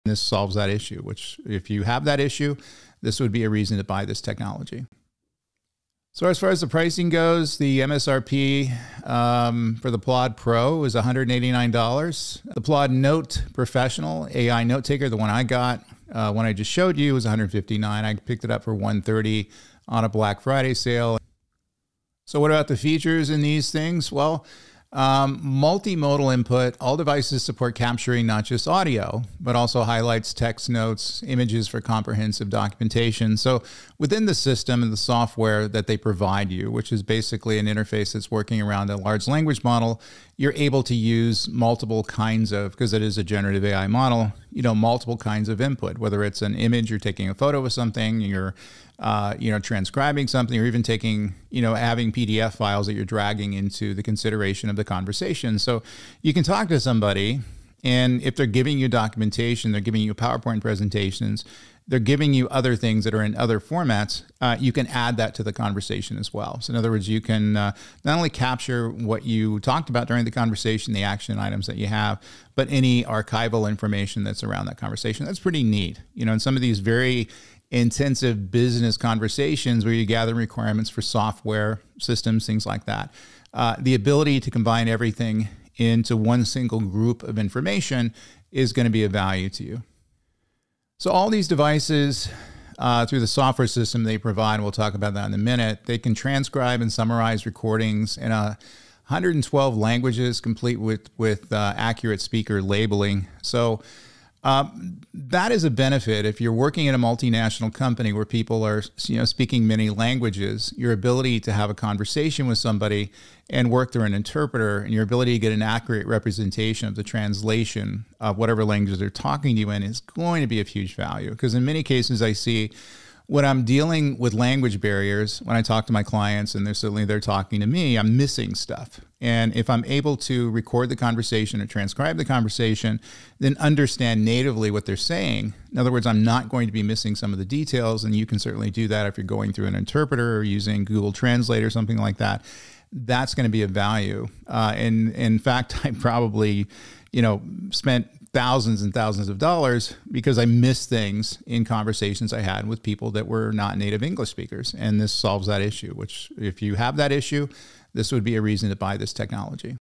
Plaud Pro AI Note-Taker: Multilingual Transcription & Summarization
Solve communication gaps with Plaud Pro! This AI note-taker transcribes & summarizes in 112 languages, capturing audio, text, images. Perfect for multinational teams needing accurate interpretations. Get it from $130!